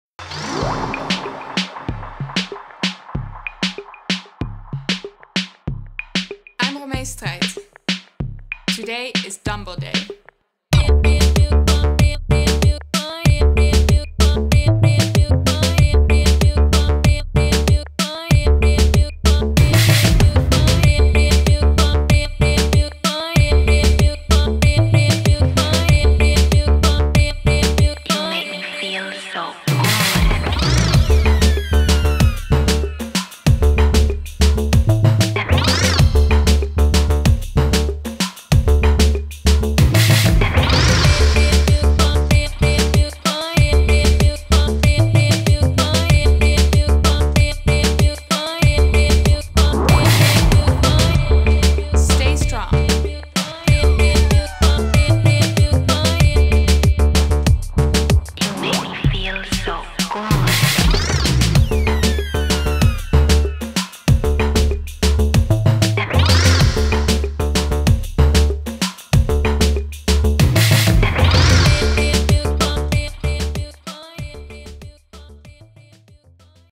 23rd December | Romee Strijd by Phil Poynter | Love Advent 2017
Day 23 of LOVEADVENT is weights day with Romee Strijd, STAYSTRONG.🏋️‍♀️

“What I like about the LOVE Advent Calendar is that they want to showcase women in a sensual perspective, but they do it in a cool, edgy way. The videos are sometimes over the top, showing a bit more of the girls than you regularly see in videos, but they get away with it, as they have a great team of creative people that put all the pieces together in a certain way that eventually makes the videos so cool.”